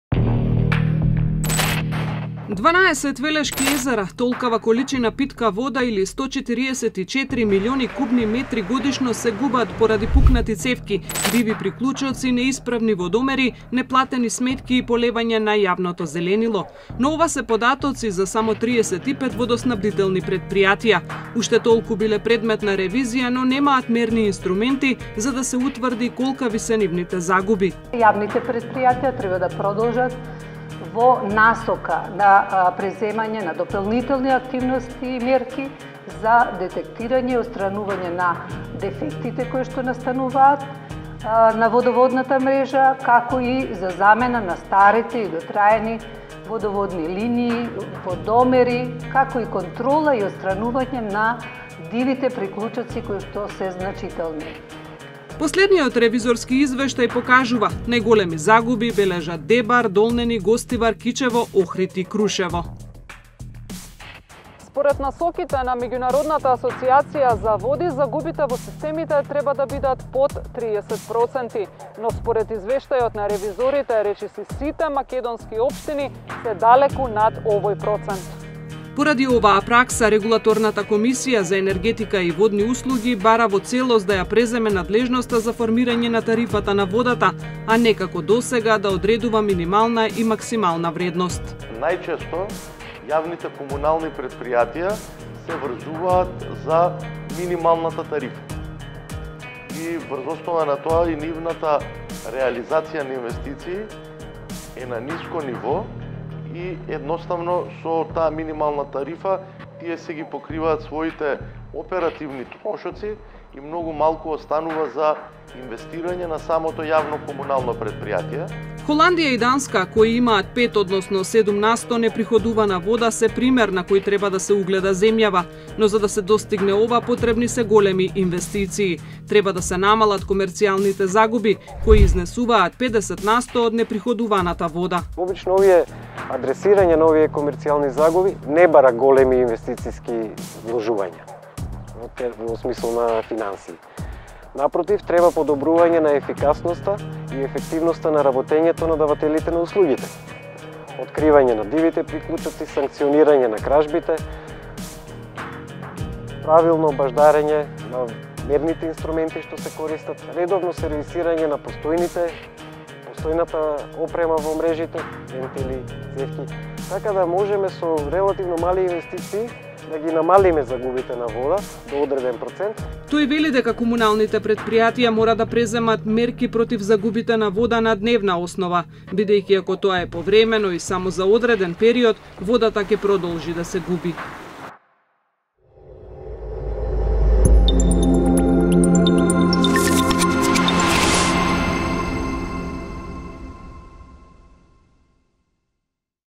Општините под итно да инвестираат против загубите на вода, алармираат експерти
Регулаторната комисија за енергетика и водни услуги бара во целост да ја преземе надлежноста за формирање на тарифата на водата, а Државниот завод за ревизија и експерти сметаат дека е нужно подобрување на ефикасноста и ефективноста на работењето на комуналните претпријатија.